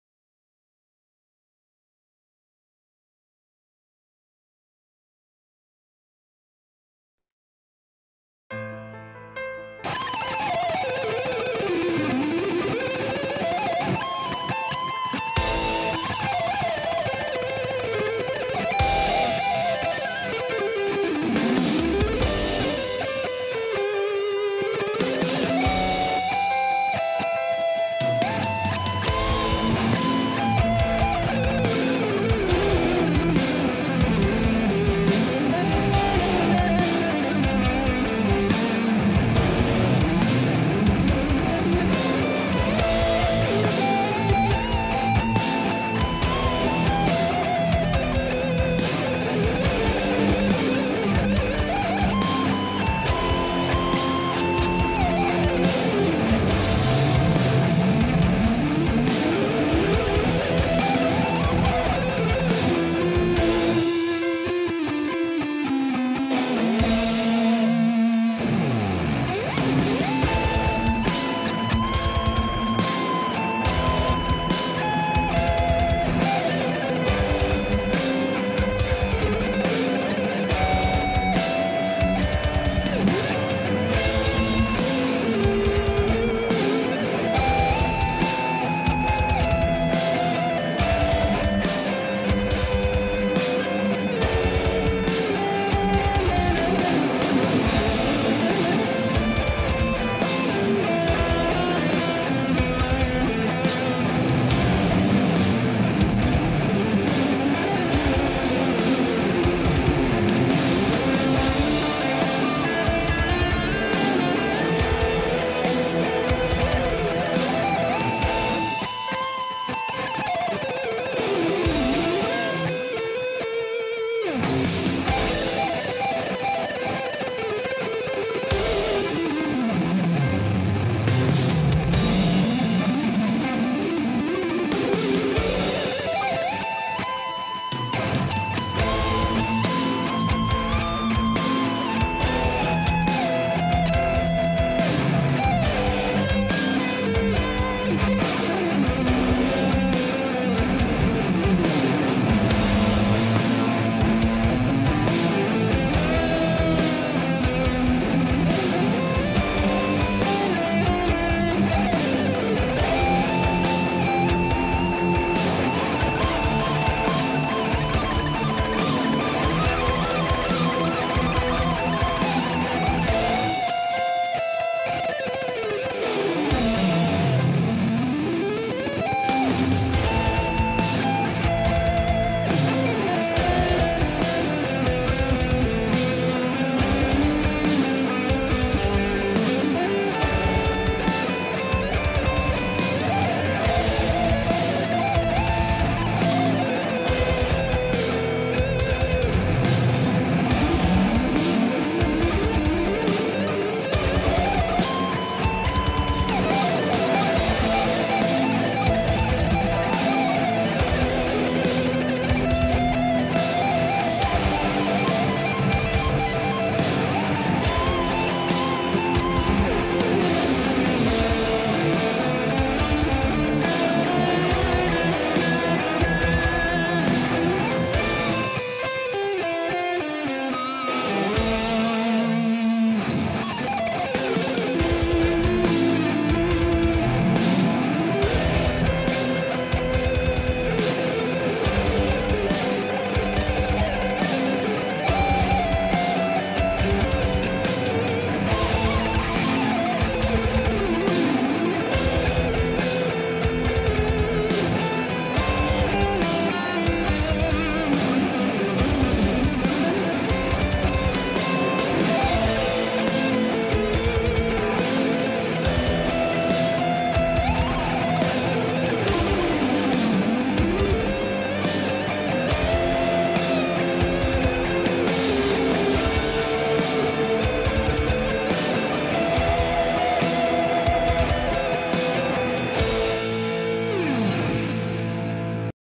Ibanez Rg8- electric guitar improvisation
A new impro with my new axe 8 strings, i hope you like it!
Una nuova improvvisazione con la mia nuova chitarra 8 corde, spero vi piaccia

Guitar: Ibanez Gr8 -Bk